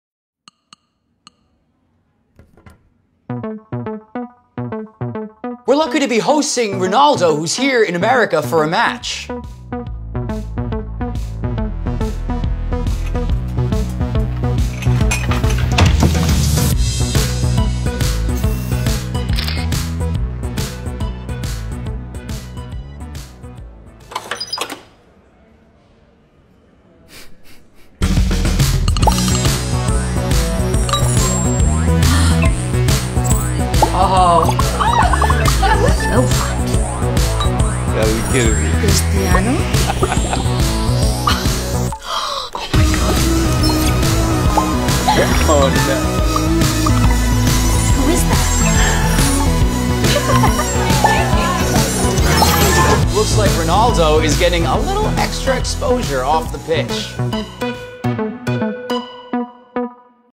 Altice - Cristiano Ronaldo
Cristiano Ronaldo in Underwear

Agency: In-house
Staring: Cristiano Ronaldo
Released: July 2017